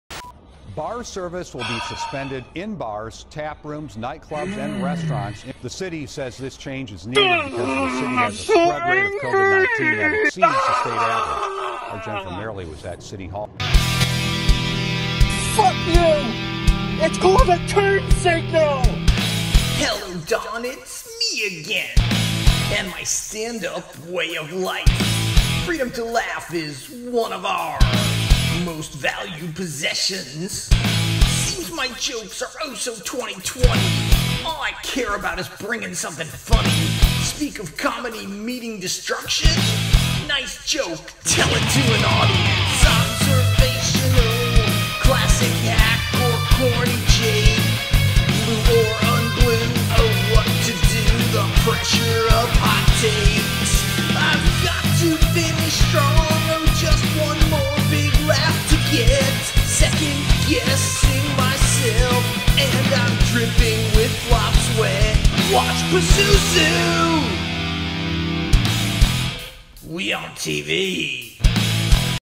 When comedians find out the clubs are closing...again
MEGADETH MEETS PUSUSU

What happens when comedians discover that the Govs Comedy Club Patio has just been closed down?

Special thanks to Andrew Plagainos and Ken Peschel!